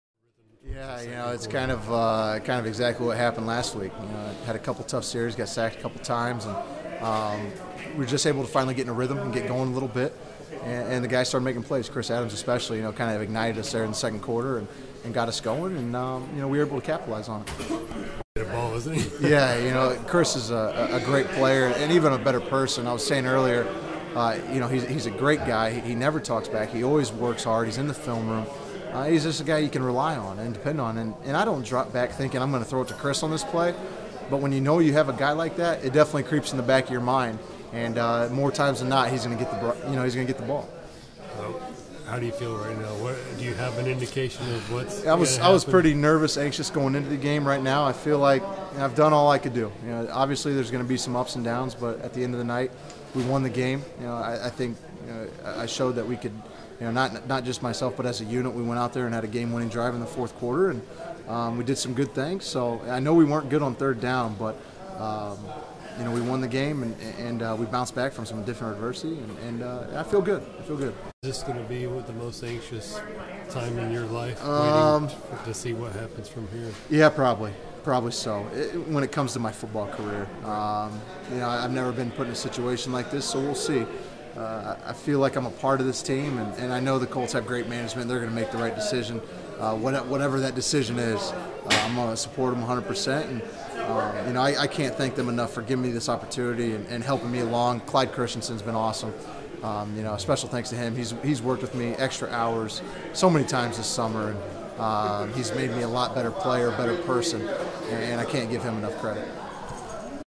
Colts QB Chandler Harnish after final preseason game
Colts rookie QB Chandler Harnish gives his thoughts as he waits to see if he makes the 53-man roster, Aug. 30, 2012